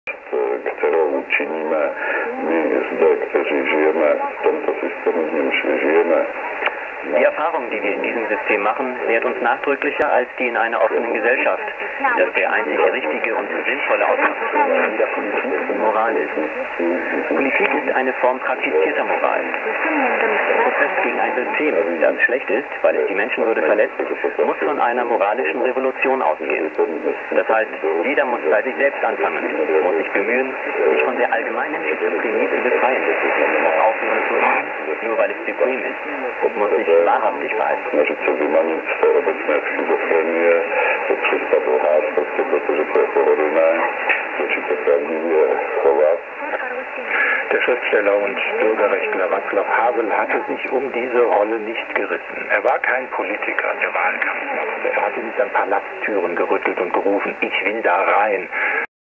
Deutschlandfunk 6190 kHz. 18.12.2011.
6190 kHz 1631 Deutschlandfunk German D Berlin. Distance 1483 km.